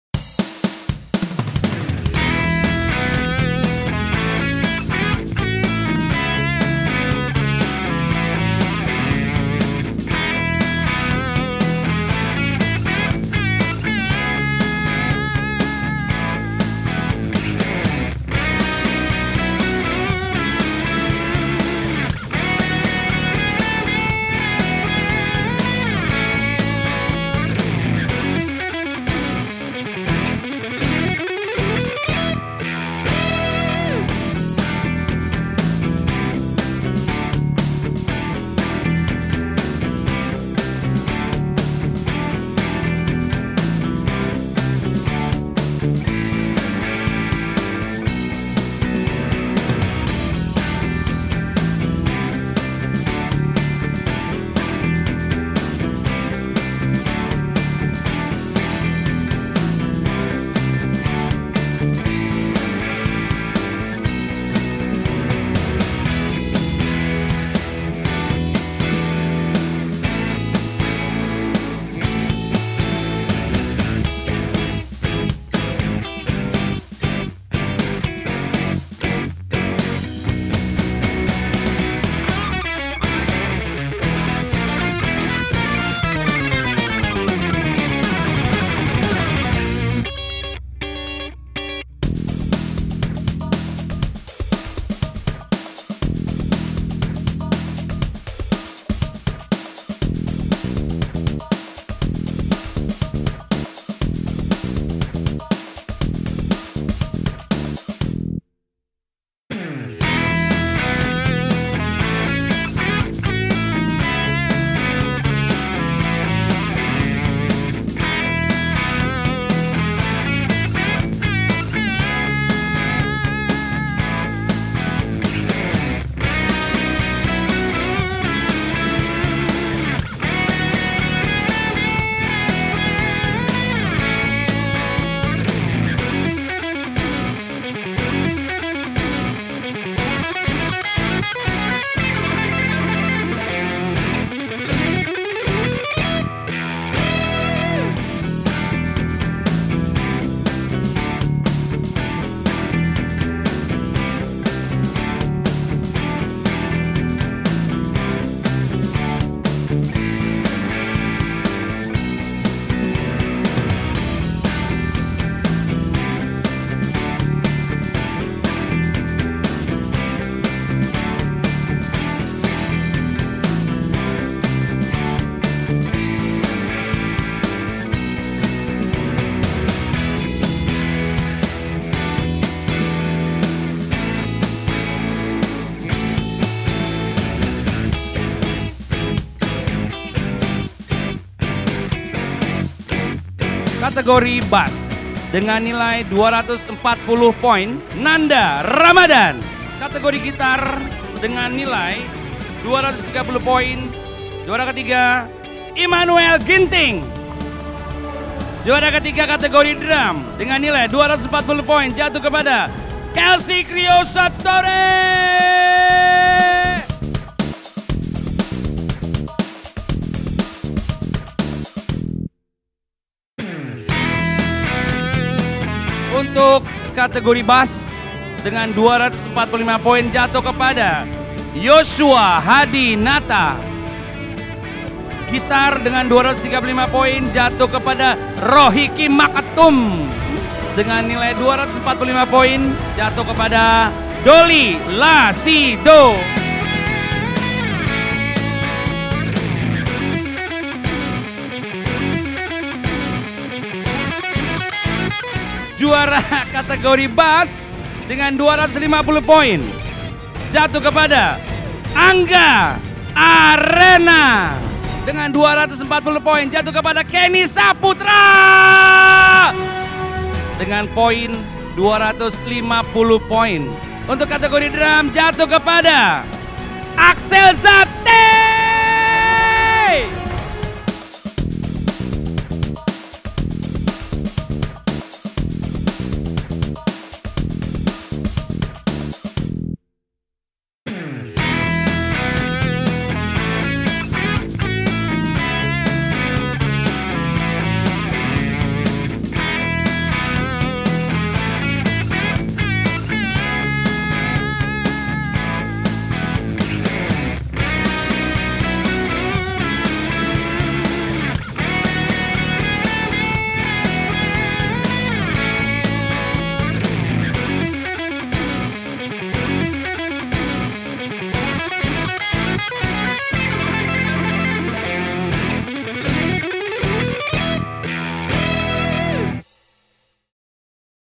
Grand Final Video Coverage - Yamaha Magnificent 7
Grand Final of Yamaha Magnificent 7 - Guitar, Bass, Drum Competition 2017 in Taman Ismail Marzuki Jakarta. 21 finalist from all over Indonesia shows their best skills in the stage.